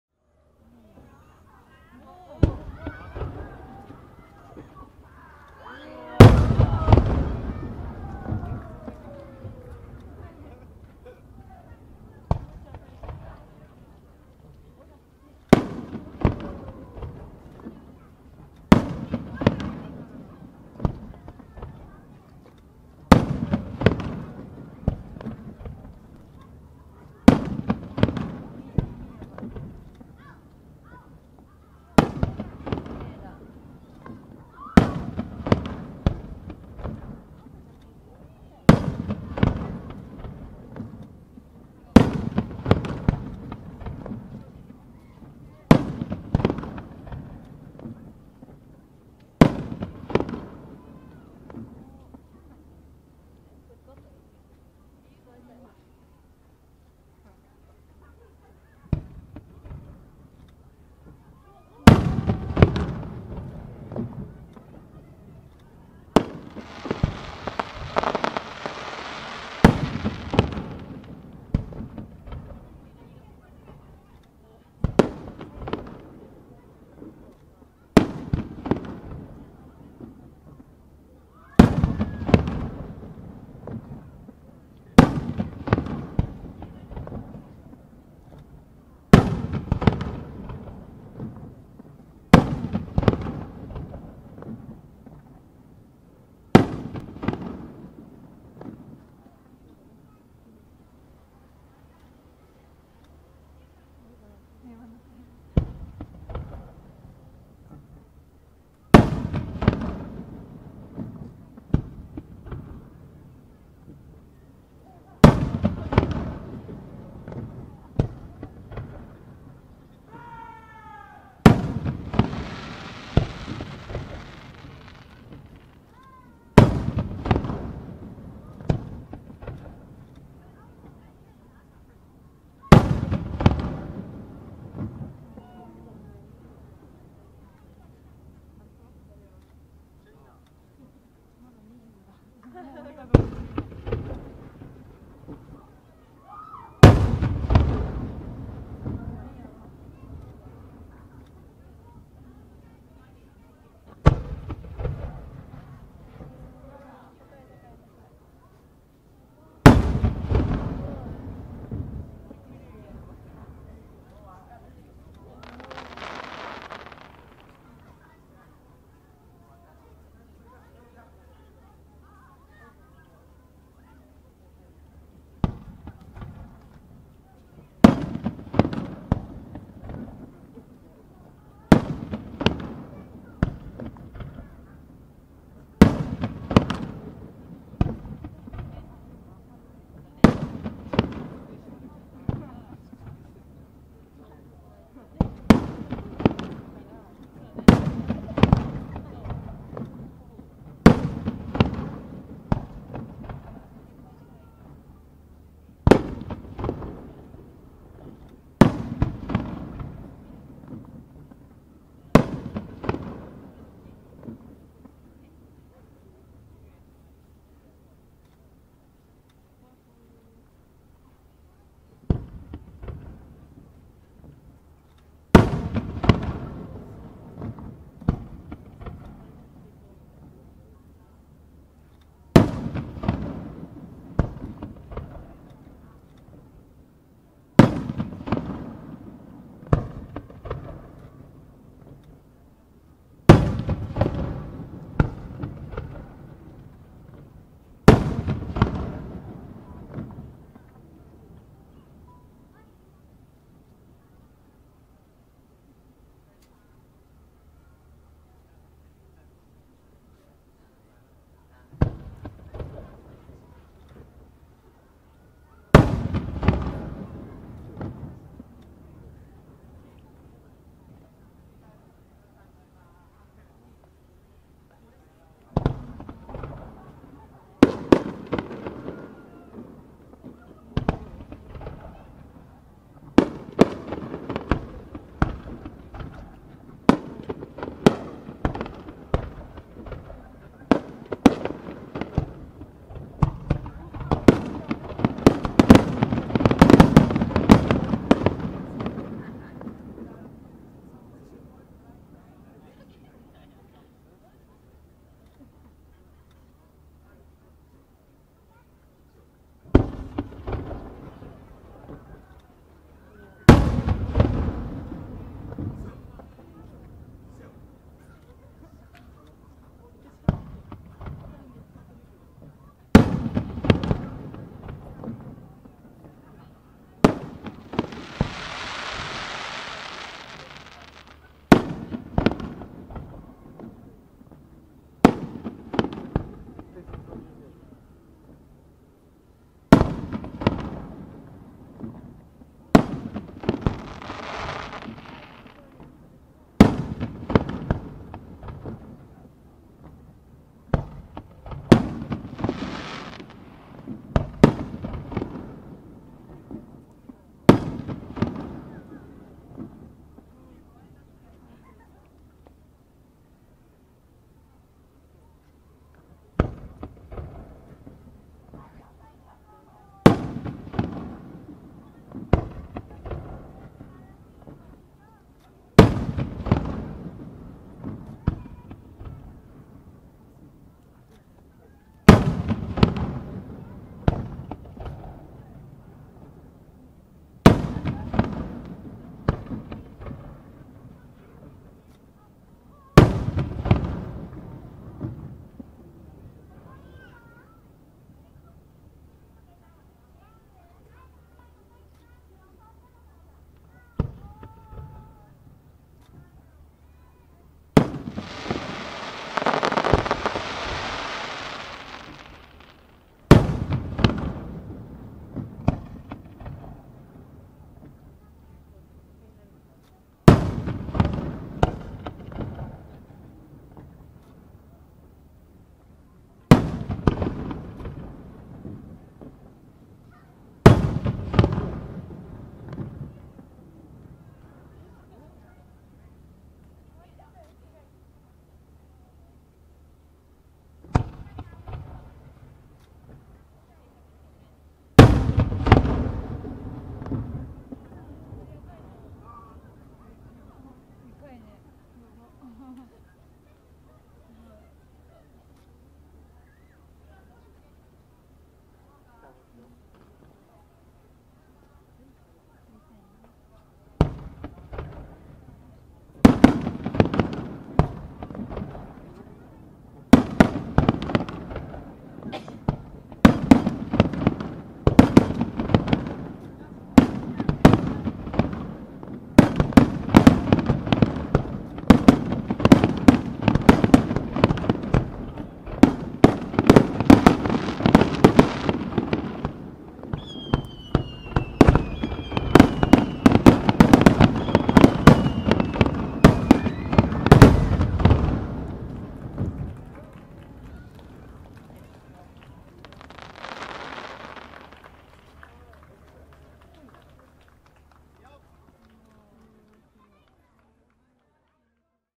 【2021年8月12日㈭】片山津温泉 納涼花火祭り【Katayamazu Onsen Fireworks Festival】
2021年8月12日㈭
石川県加賀市「片山津温泉」にて2021年8月1日から29日まで毎日打ち上げられている花火